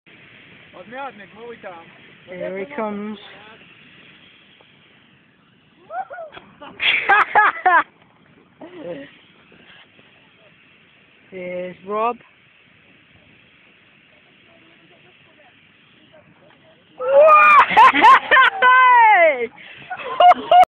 Ski Air Bag
Couple of my friends first attempts on a ski air bag, the first guy is em and the second is rob, his excuse for doing a half backflip was that he didnt want to land on his face =)

Filmed in Italy, Dolomites , Belvedere bowl, a short cable lift from canazei